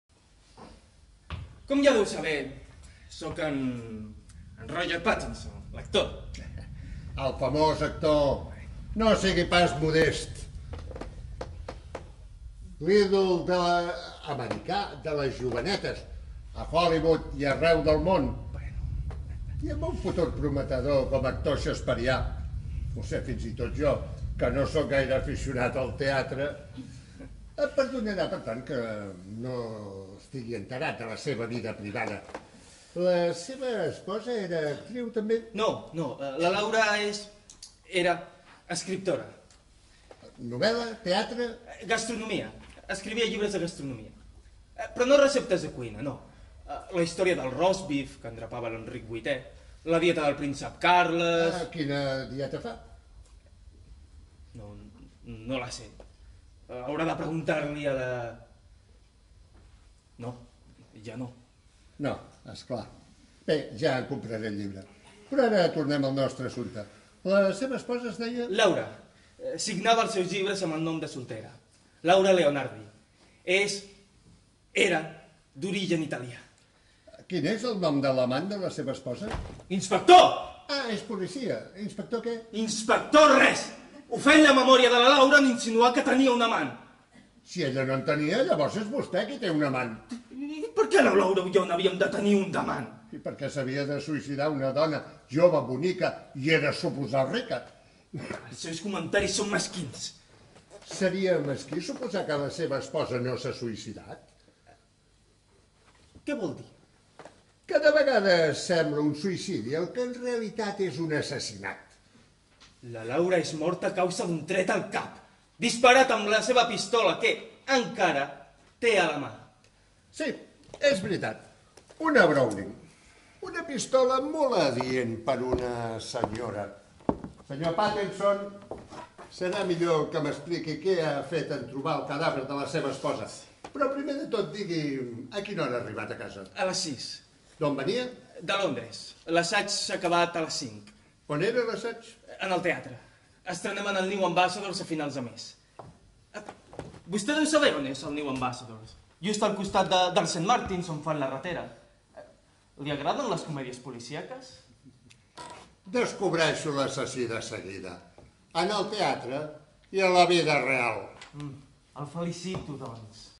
Cadàver? (1)
Cadàver?, d'Octavi Egea
Quadre escènic del CCSants
Sants Teatre. 15 de febrer de 2020